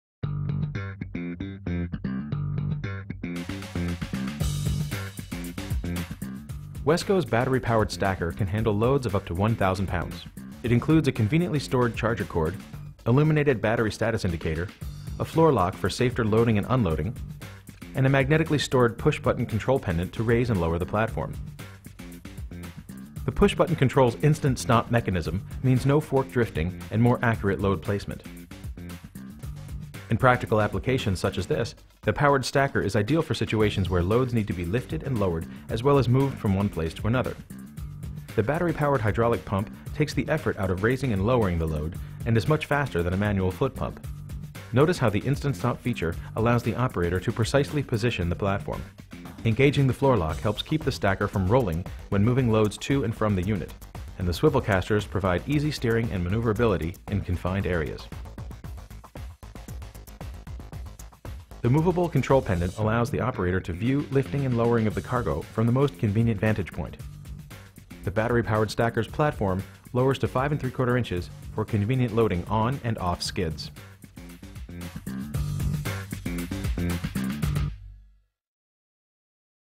Powered Stackers Platform Lift by Wesco
A great piece of equipment perfect for your warehouse. Available in 6 different models. Up to 80" lift height, 1,000 pound capacity.